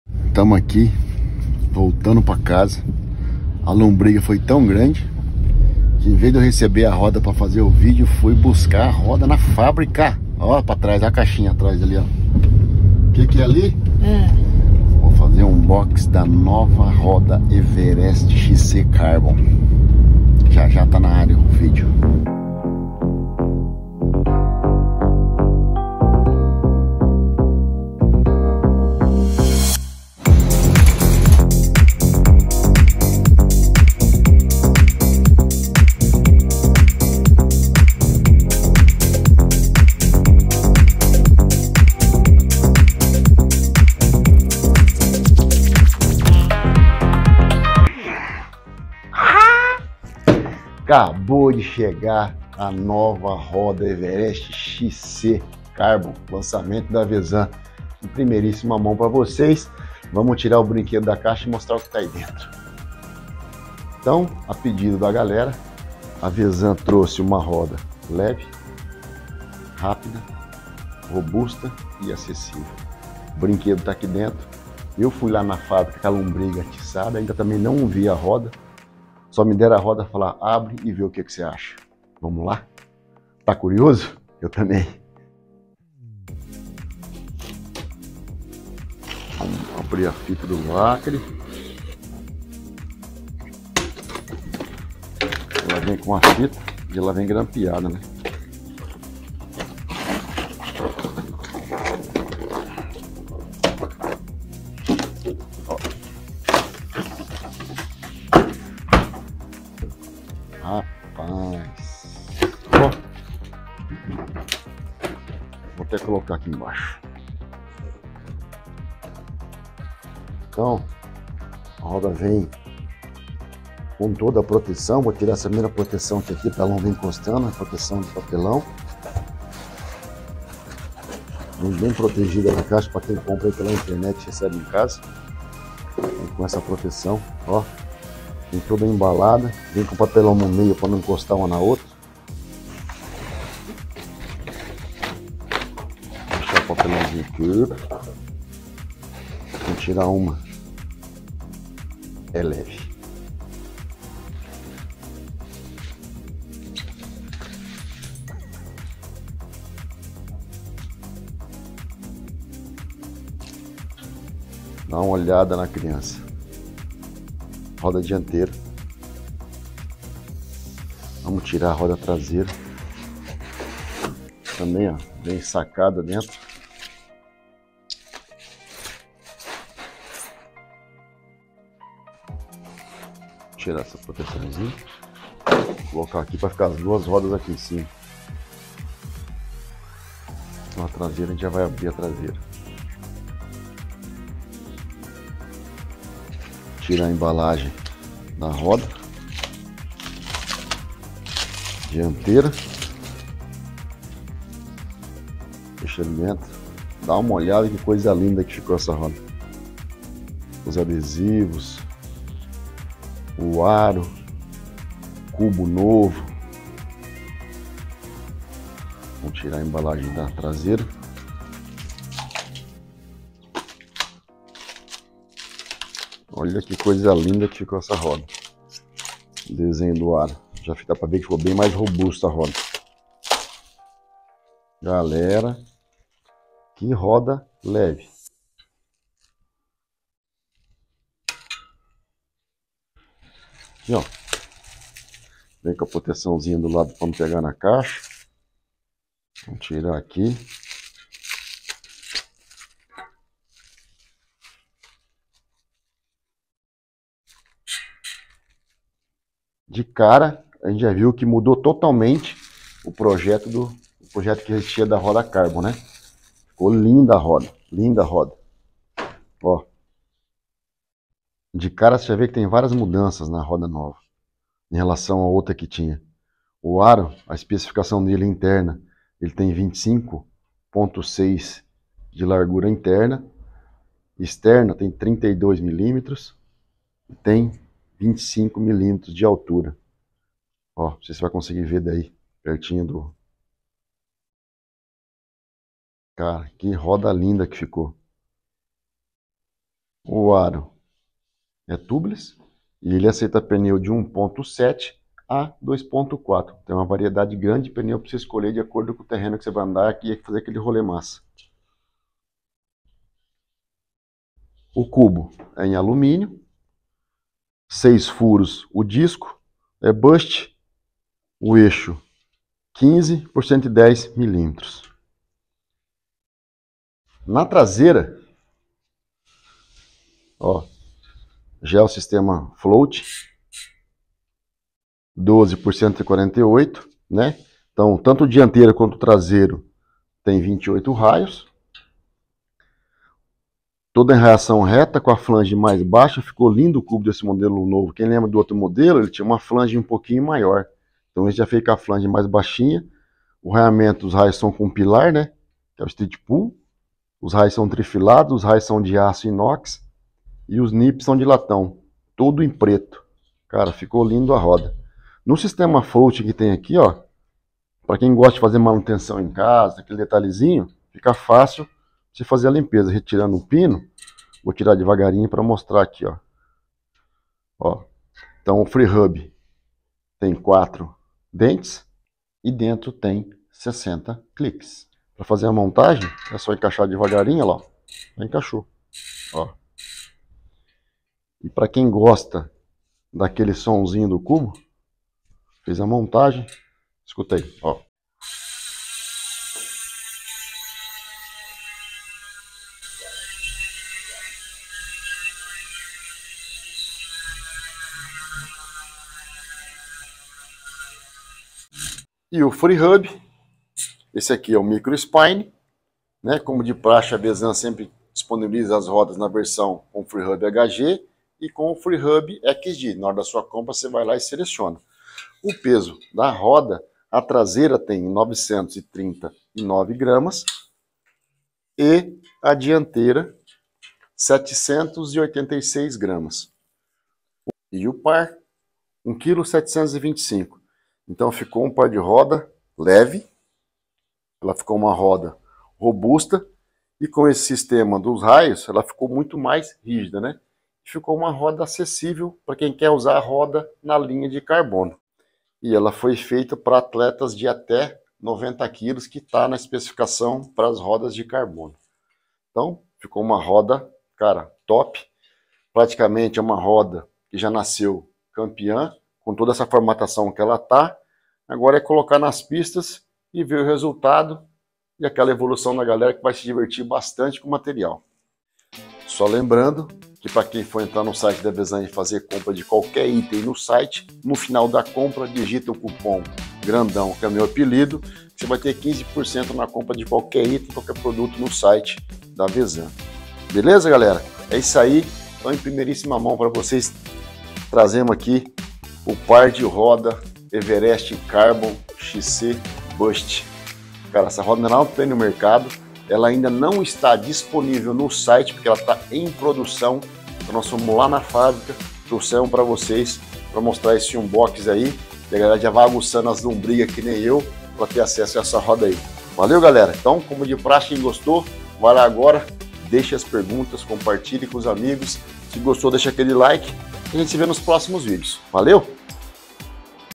UNBOXING DA RODA VZAN EVEREST CARBON MTB UMA OPÇÃO INCRÍVEL PARA UM RODA ROBUSTA LEVE E RÍGIDA
A nova roda VZAN Everest Carbon Boost veio para atender aos pedidos dos consumidores que queriam uma roda leve, rápida, robusta, acessível, e que já nasceu campeã!
Equipada com aros em fibra de carbono, com 25,6mm de largura interna, o que lhe permite utilizar pneus 1.7 a 2.4, compatível com pneus tubeless. Cubos em alumínio com quatro rolamentos, freehubs com quatro dentes e 60 clicks, e como sempre, as três opções de freehub, Shimano HG, Shimano Microspline, Sram XD, que melhor se adaptam ao seu kit relação. Raios Pillar straight pull trefilados de 1.8mm, visando leveza sem perder rigidez.
Tudo isso com a garantia e qualidade VZAN!

Cor: Preto matt

RODA
QUANTIDADE DE RAIOS 28 raios na dianteira e na traseira
ROLAMENTOS Dianteira (2)18307 e traseira CUBO 6903
FREEHUB Traseiro SHIMANO HG
17287
SHIMANO MICROSPLINE 6802
SRAM XD 6803

TAMANHO DOS RAIOS
DIANTEIRA 298MM LADO DO DISCO / 300MM LADO OPOSTO
TRASEIRA 298MM LADO DO DISCO / 297MM LADO DO FREEHUB

ARO
Fibra de carbono – 29” X 25,6mm – Perfil com 25mm de altura
CUBO DIANTEIRO Modelo MTB FRSP BOOST
FREIOS Disco padrão seis furos
VÁLVULA Fina (Presta) 6,5mm
CUBO TRASEIRO Modelo MTB FRSP BOOST 148
PESO
Roda dianteira: 786gr
Roda traseira: 939gr

Par: 1725gr (raios flat sem acessórios)
TIPO DO PNEU
Tubeless – 29” X 1.7” A 2.5”
BLOCAGEM
N/A
ACESSÓRIOS
Duas fitas de aro
RAIOS
Pillar Cilíndricos 1.8mm STP em Aço Inox Preto
FREEHUB
Shimano HG – 8 a 11 velocidades
Shimano Microspline – 12 velocidade
Sram XD – 12 velocidades

NIPLES
Latão Preto 16mm

SISTEMA DE TRACIONAMENTO
Floating – 4 dentes e 4 molas

*Recomendado a atletas até 90kg
*Peso informado é referente as rodas sem acessórios
*Podem haver variações de até 5% no peso do produto.
*A VZAN se reserva ao direito de alterar as características do produto sem aviso prévio.

Quer interagir conosco e entrar no nosso Grupo de Whatsapp?
Entre ai